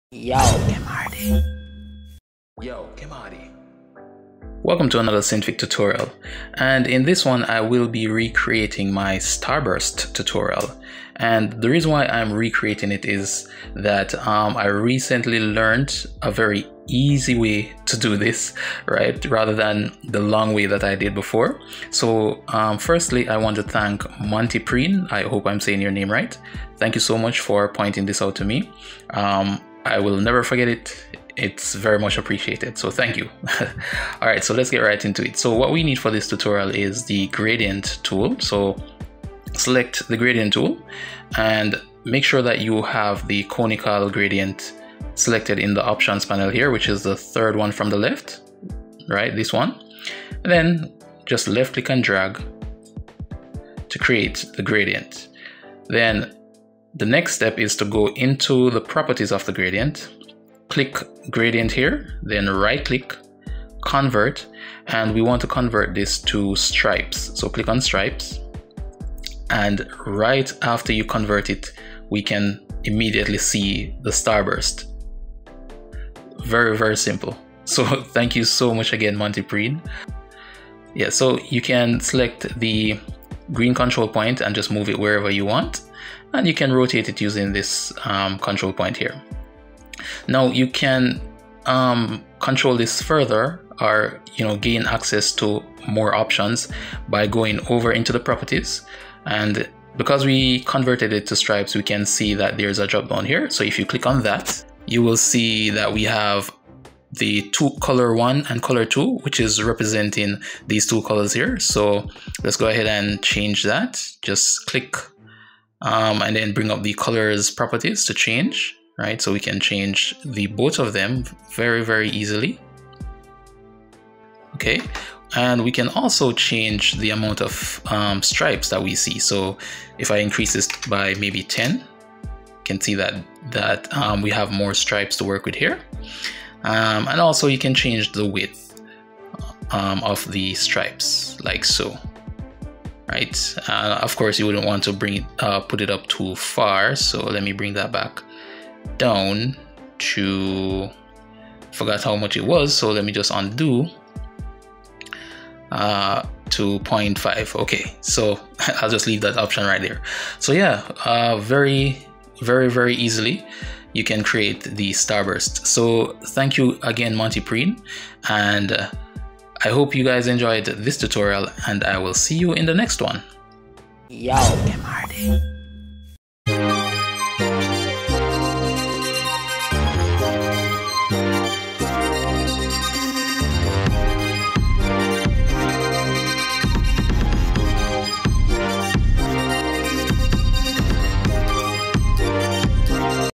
Synfig for beginners: 49 - Creating a Starburst (The Easy Way)
This is an introductory series into Synfig. The version used in this video is 1.4. Synfig is an opensource 2D animation software with many powerful tools and features. It's still being developed but with each update, it gets better and better.

This is a second tutorial on how to create a starburst. This is an easier way of getting the task done. It was told to me by montypreen, so a big thank you! Also here's the first tutorial for those wanting to go the harder route xD